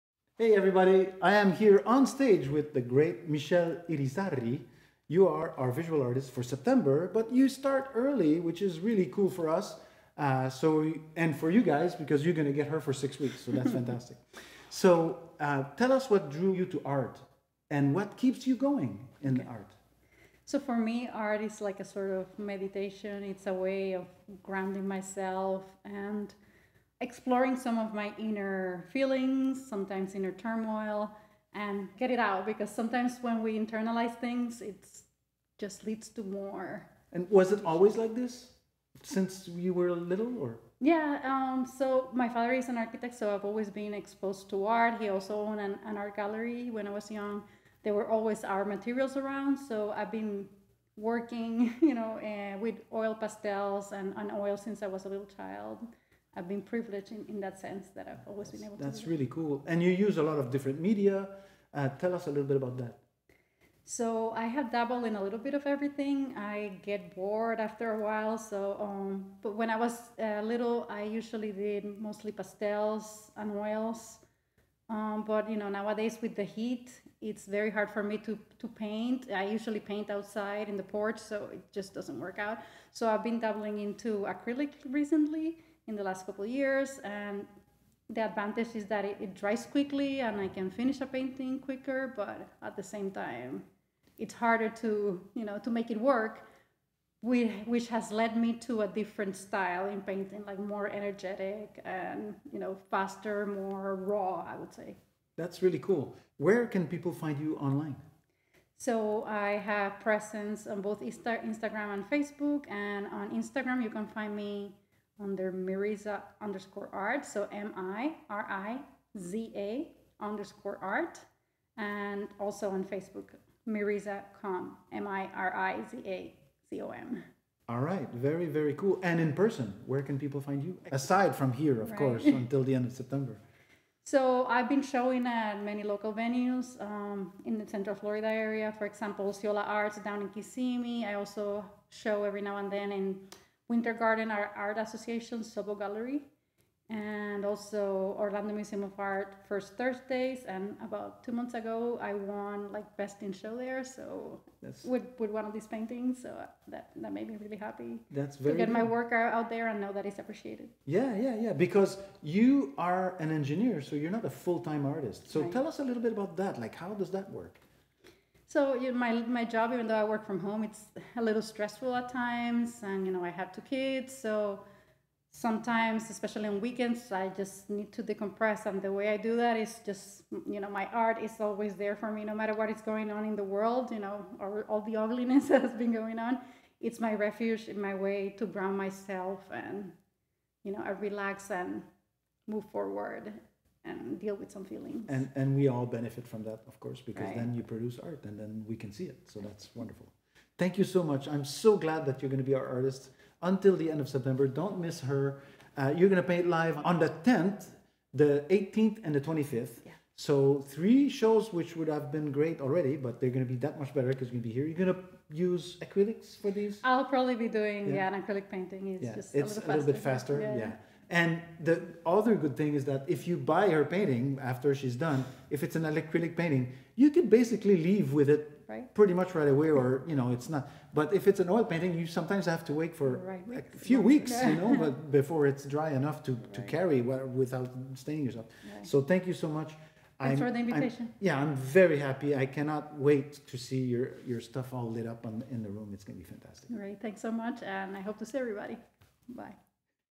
Timucua Talks with Michelle Irizarry
Michelle is our visual artist for September 2022, with a bonus few days in August :-)